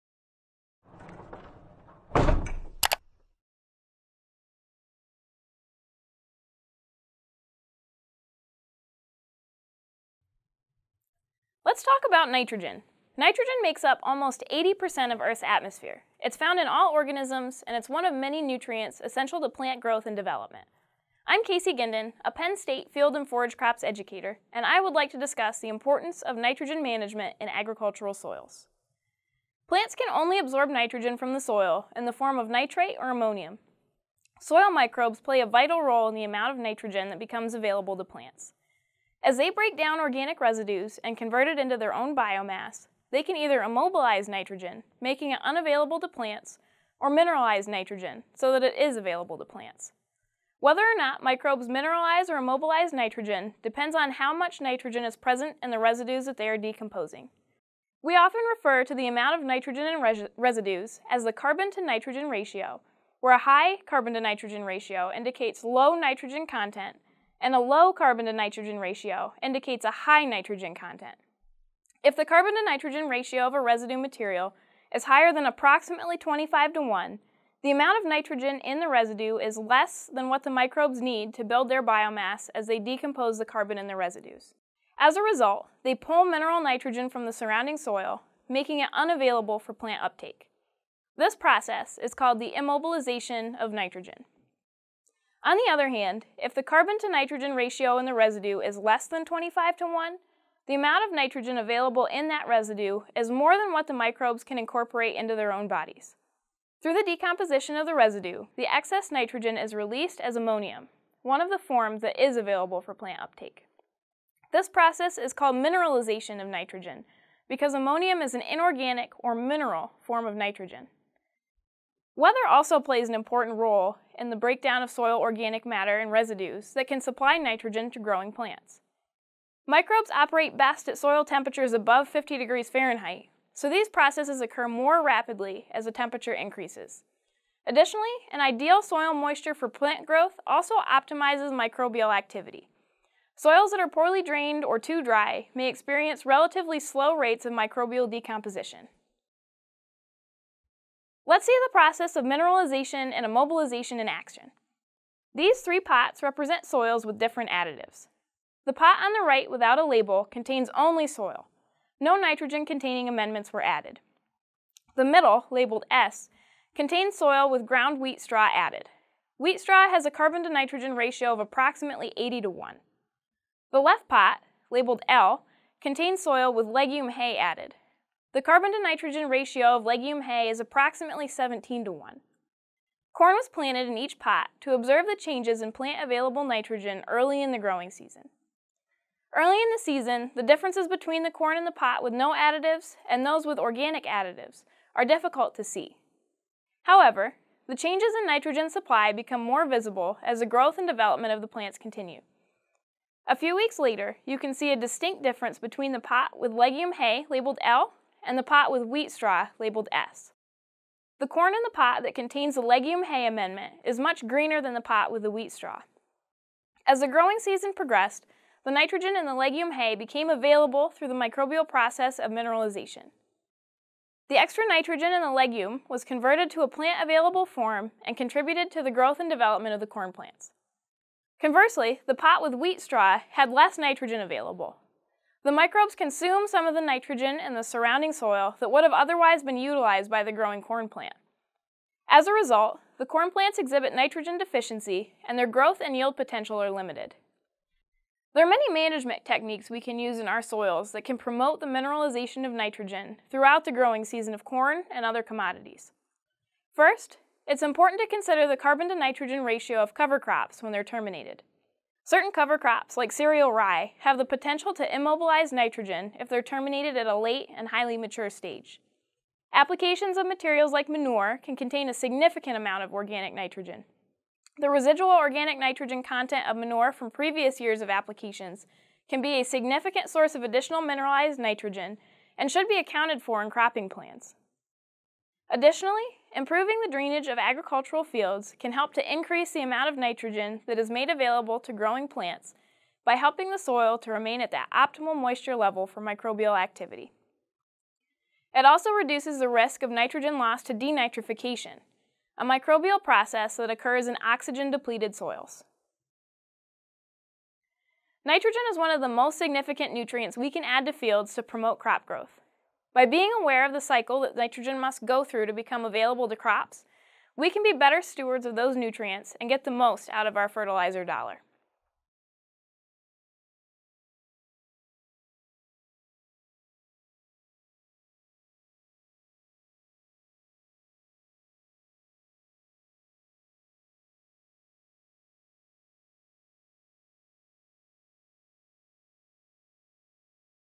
Immobilization and Mineralization of Nitrogen in Agricultural Soils
Nitrogen availability in soils is controlled by a process called the nitrogen cycle. This video explores two pieces of the cycle- immobilization and mineralization.